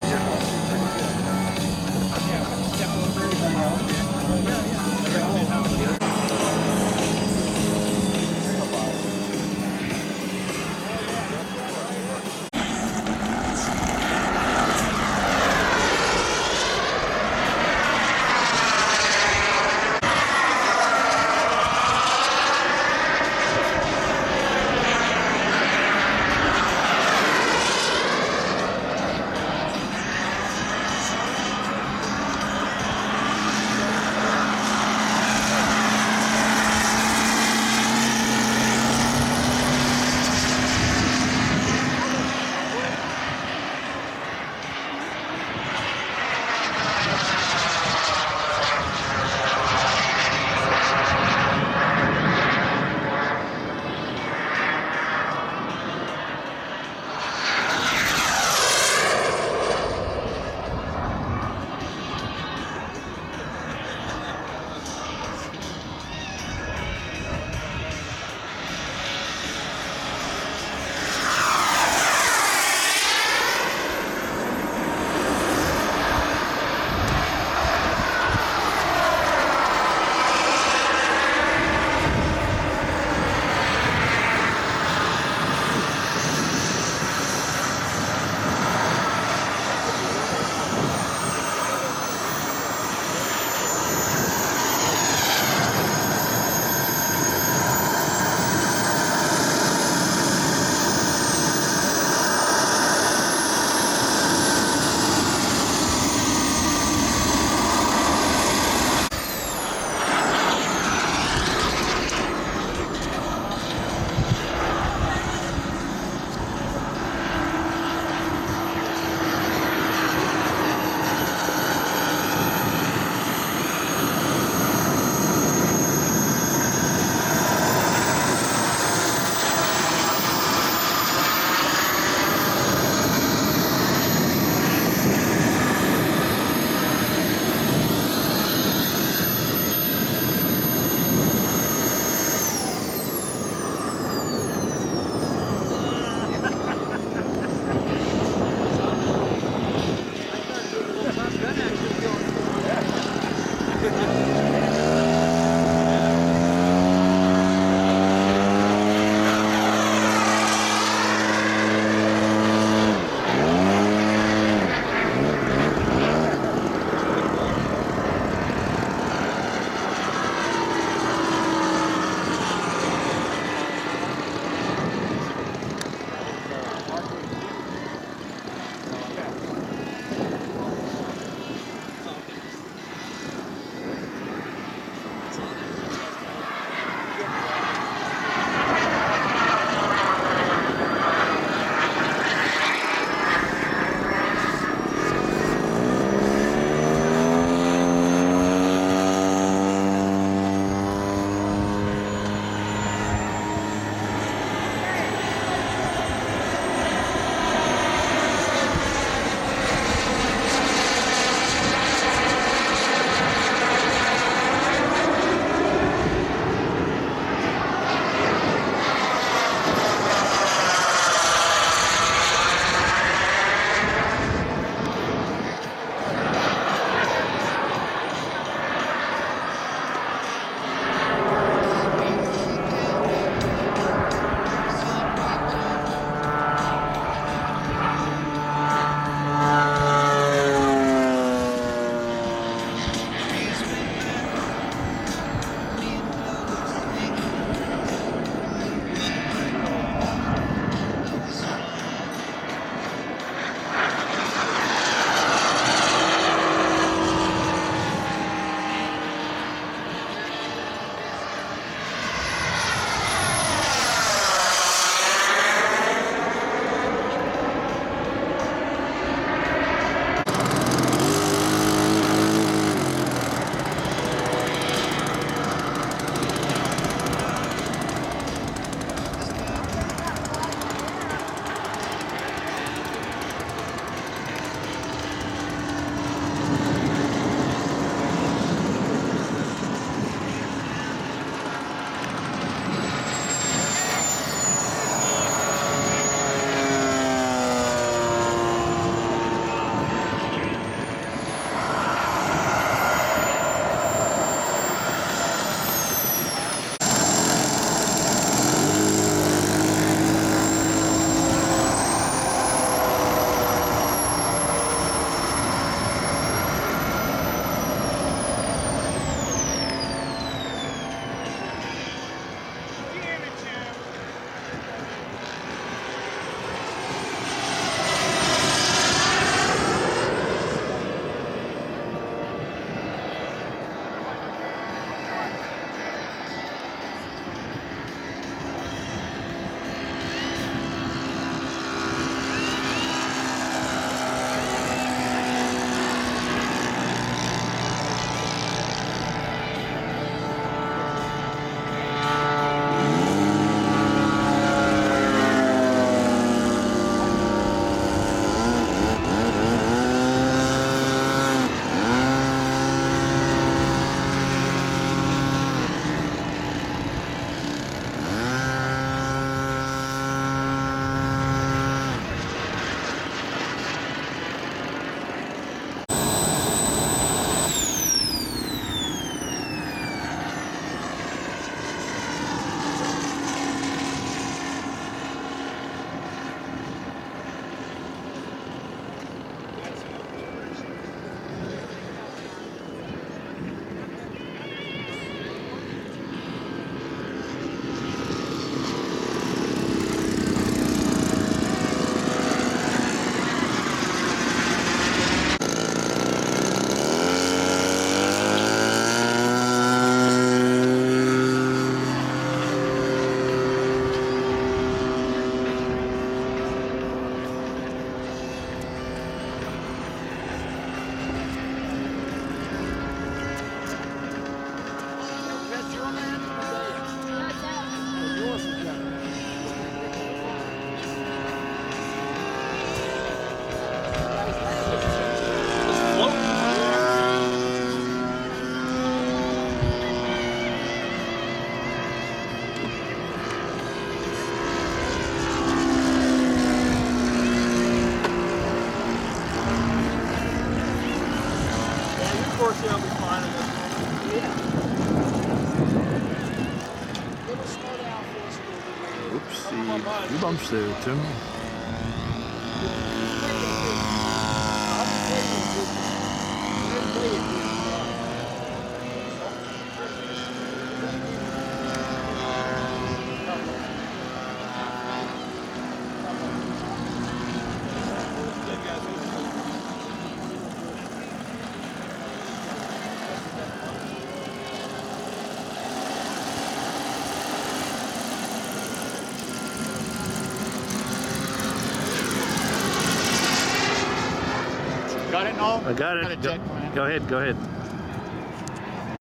Tim's PT-17 Stearman
The PT-17 is powered by a Robart Radial engine that has been converted to gas (petrol).  Hopefully Tim will include details of the model in the comments.

Unfortunately the great sound of the radial is mostly drowned by the usual sounds of an event - in this case the 2024 Winter Warbirds at SVF.